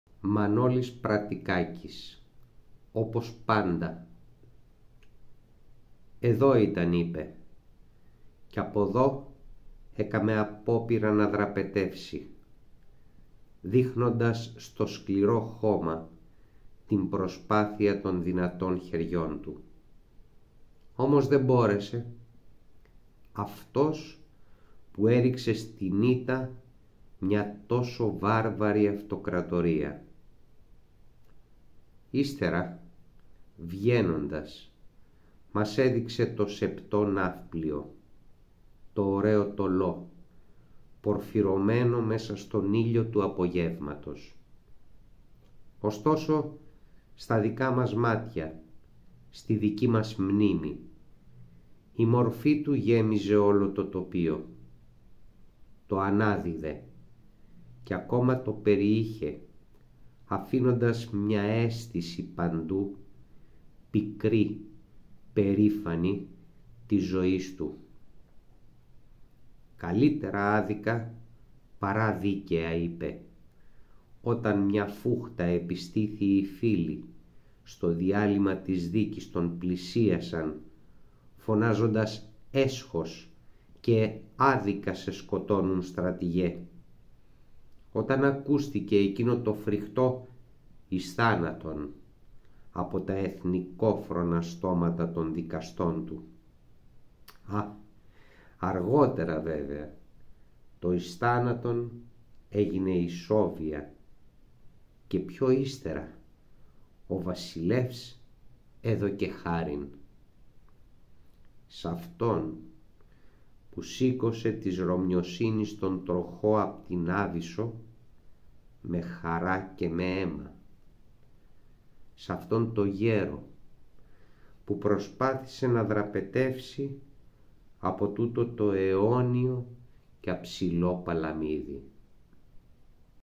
ΜΑΝΟΛΗΣ ΠΡΑΤΙΚΑΚΗΣ - ΟΠΩΣ ΠΑΝΤΑ
Μανόλης Πρατικάκης - Όπως πάντα
Ποίημα αφιερωμένο στον Θεόδωρο Κολοκοτρώνη.

Διαβάζει ο Δημήτρης Φιλελές

Για να διαβάσετε το ποίημα και πολλά ακόμη, ακολουθήστε τον σύνδεσμο